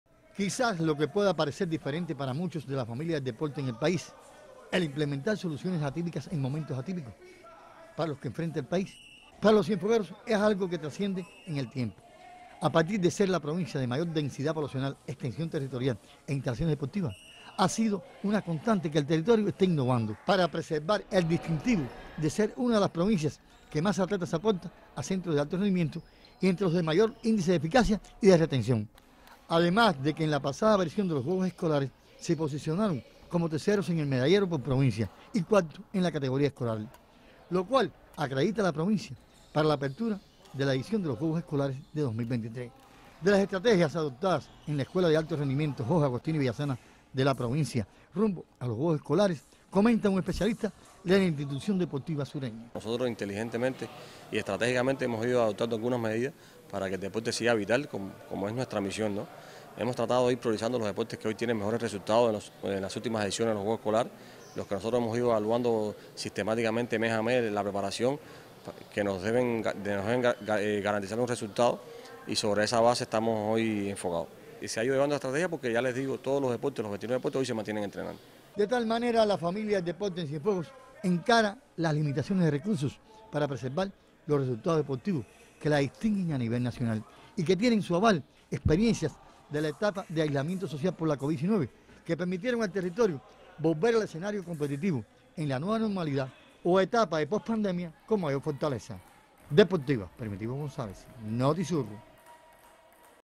Implementar adecuadas estrategias en el deporte es alcanzar resultados
En tiempos atípicos la implementación de estrategias y proyecciones de trabajo tienen su recompensa. Se trata de conservar la preparación y los resultados deportivos de los atletas.